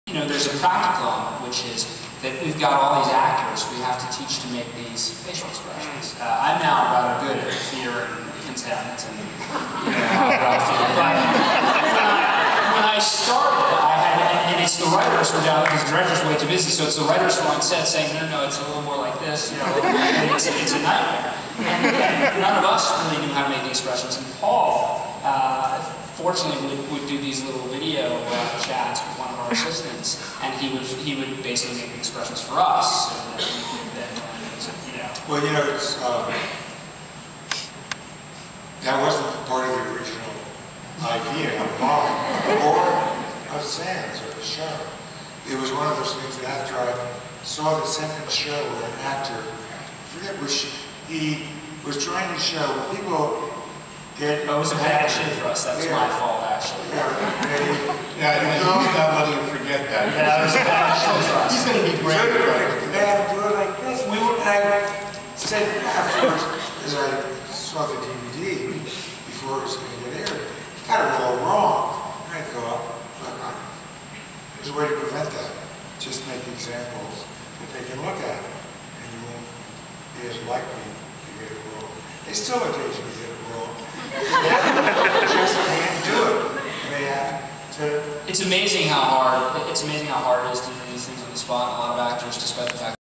APS - Paul Ekman with Lie To Me writer Josh Singer Part 1
Filmed at Association for Psychological Science (APS) Conference May 23, 2009. On translating science for television